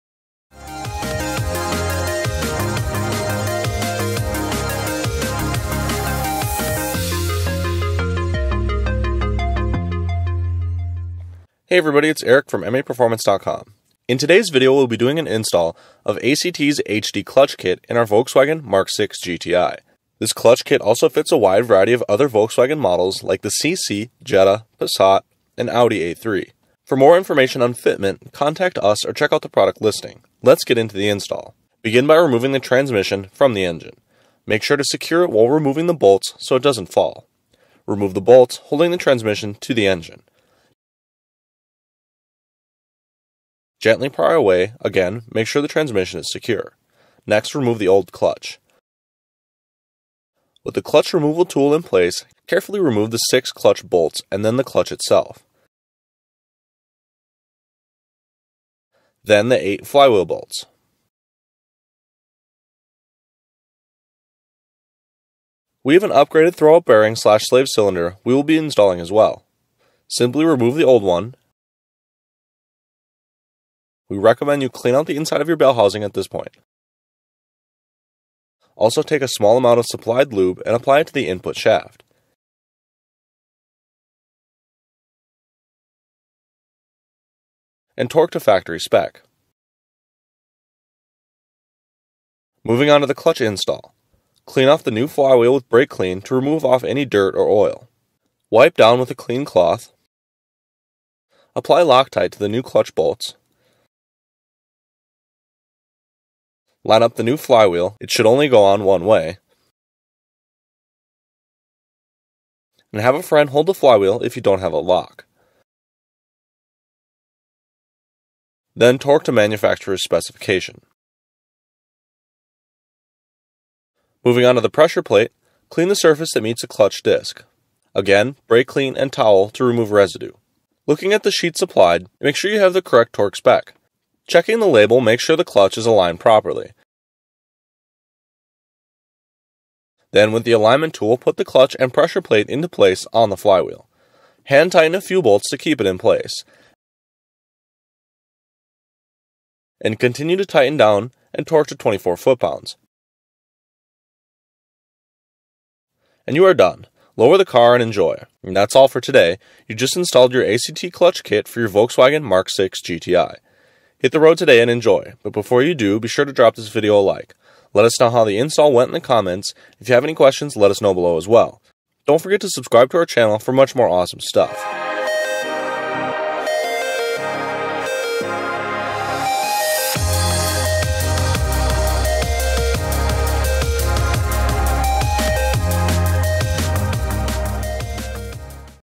Mk6 VW GTI Clutch Install | ACT VW8-HDSS
ACT’s new clutch kits for the 2.0L Turbo TSI 6 speed include a Heavy Duty pressure plate, Streetlite flywheel, a choice of disc, release bearing, and alignment tool. The Heavy Duty pressure plate incorporates hardened diaphragm fingers for decreased wear and provides a 30% increase in clamp force over the stock unit.